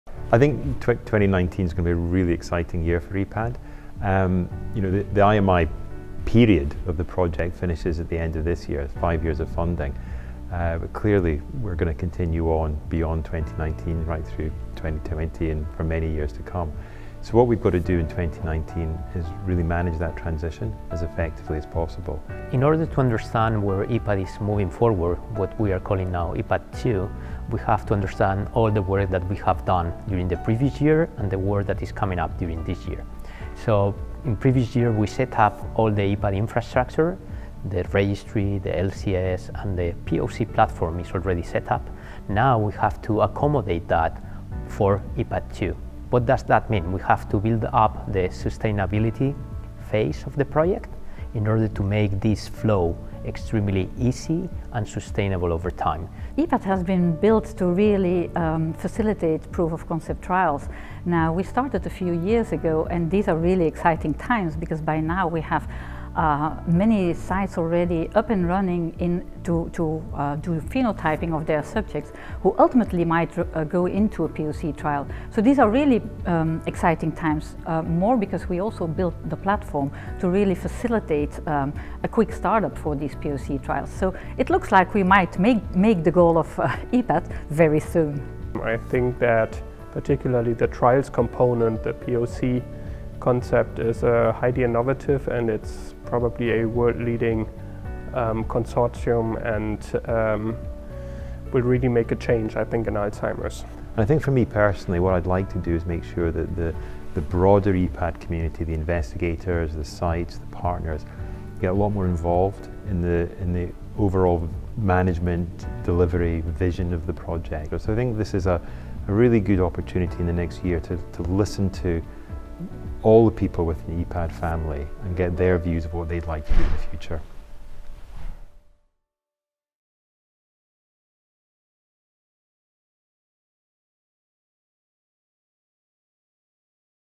EPAD Goals 2019
Craig Ritchie, José Luis Molinuevo, Katrin Haeverans and Frank Jessen explain the goals of the EPAD project for 2019.